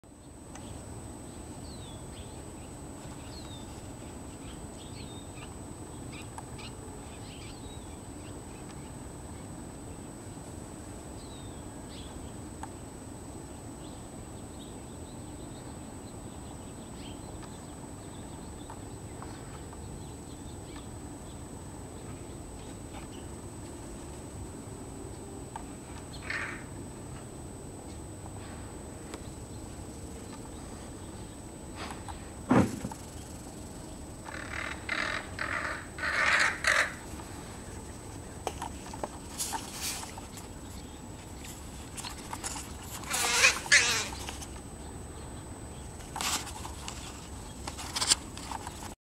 Winged Combat: Crows versus Hawk
A gang of crows drives a Coopers Hawk from my backyard.